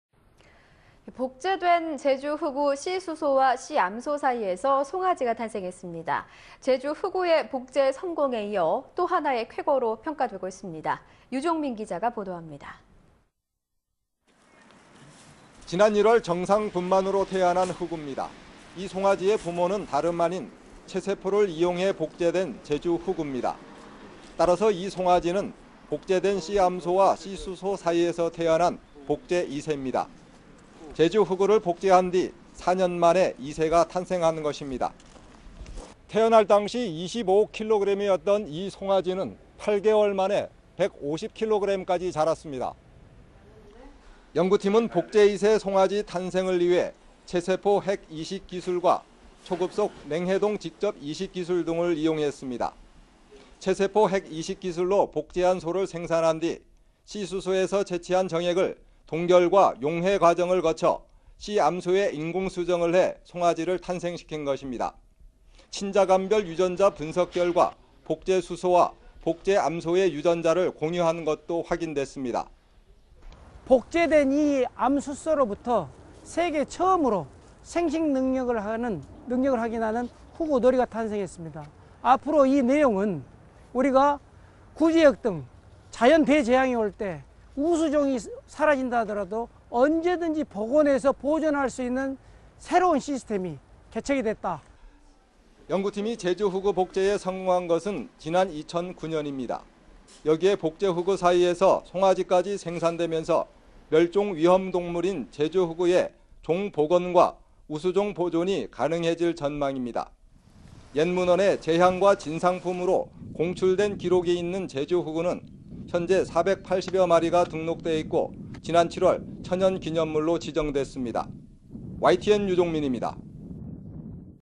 복제 제주 흑우 씨암·수소에서 송아지 탄생! / YTN 사이언스
[앵커]복제된 제주 흑우 씨수소와 씨암소 사이에서 송아지가 탄생했습니다.제주 흑우의 복제 성공에 이어 또 하나의 쾌거로 평가되고 있습니다.유종민 기자의 보도입니다.[기자]지난 1월 정상 분만으로 태어난 흑우입니다.이 송아지의 부모는 다름 아닌 체세포를 이용해 복제된 제주 흑우입니다.따라서 이 송아지는 복제된 ...